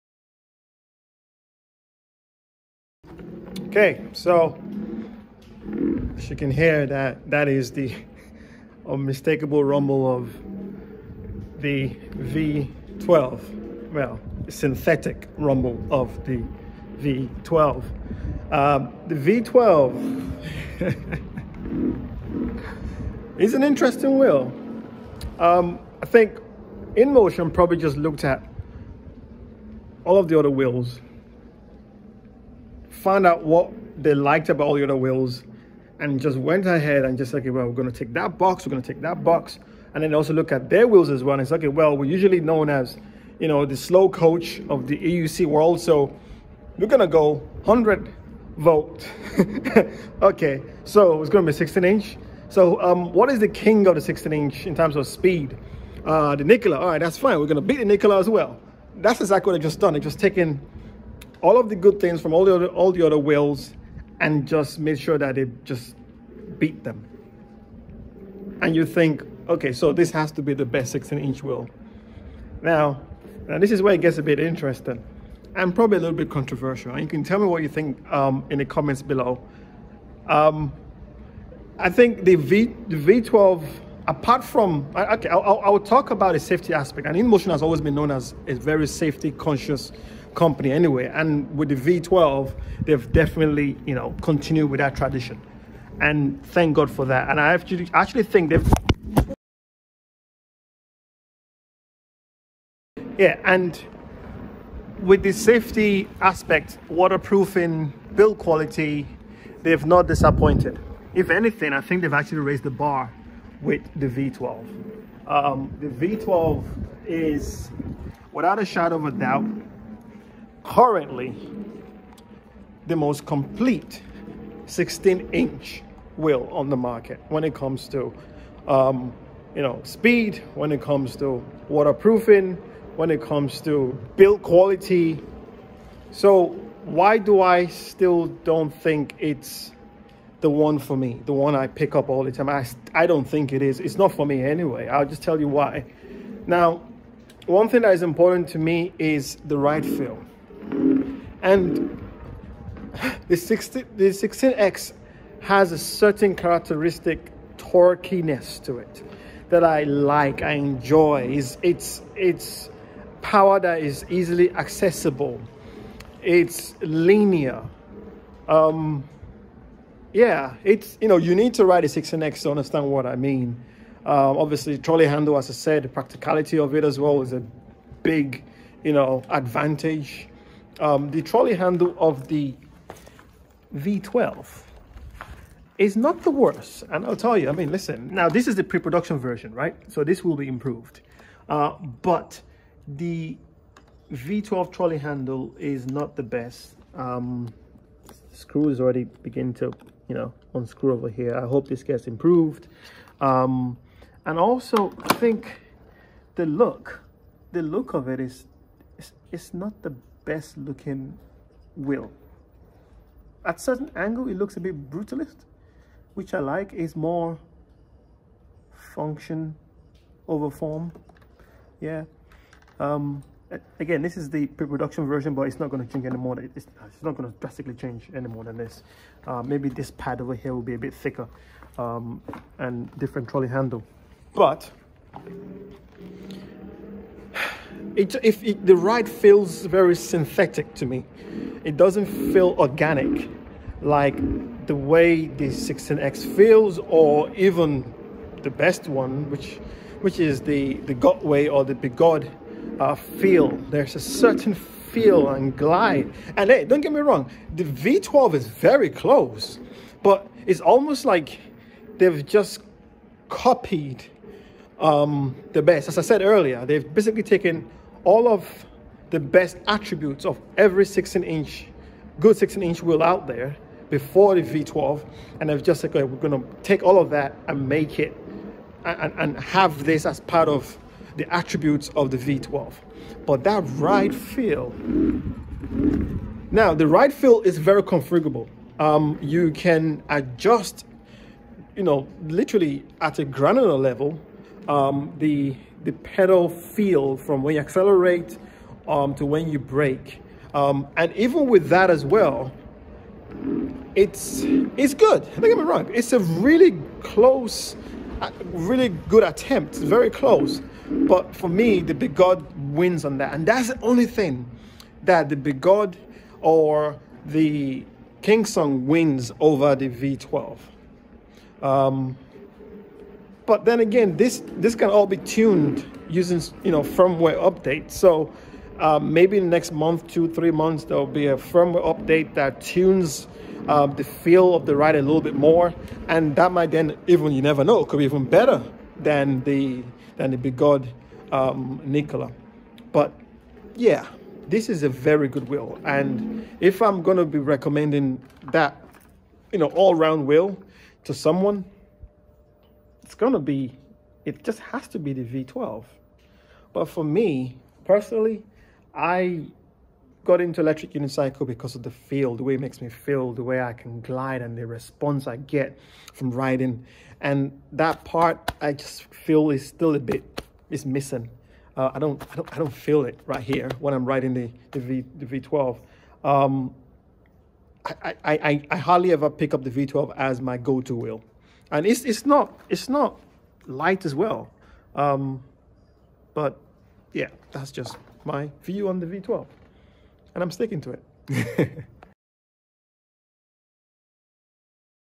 Inmotion V12 vs Begode Nikola 100v vs Kingsong 16X: ‘My’ VIEW
I share my personal experience and feedback on the Inmotion v12 compared to the Kingsong 16X and the Begode Nikola 100v. Things may change as new updated firmware roll out. So I am happy to keep an open mind.